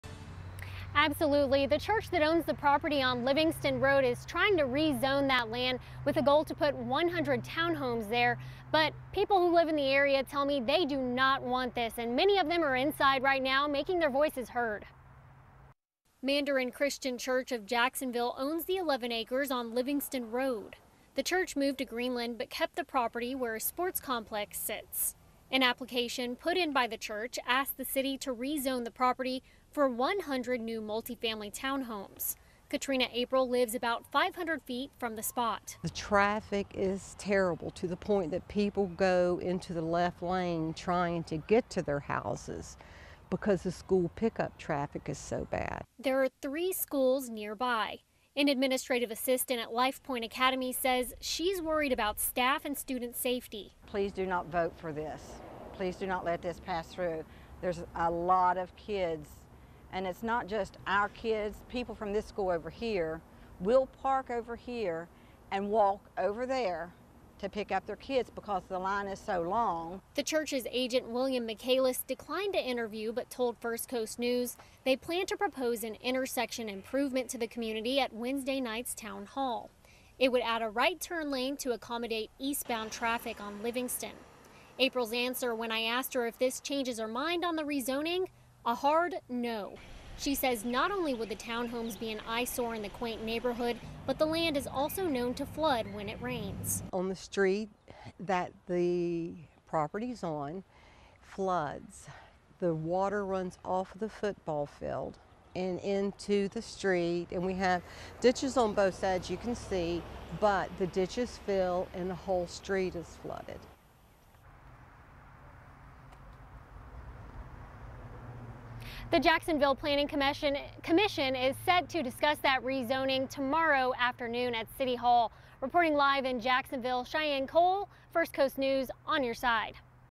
Mandarin neighbors speak out against rezoning proposal ahead of town hall meeting
A town hall will take place Wednesday at 6 p.m. to discuss the issue, open to the public.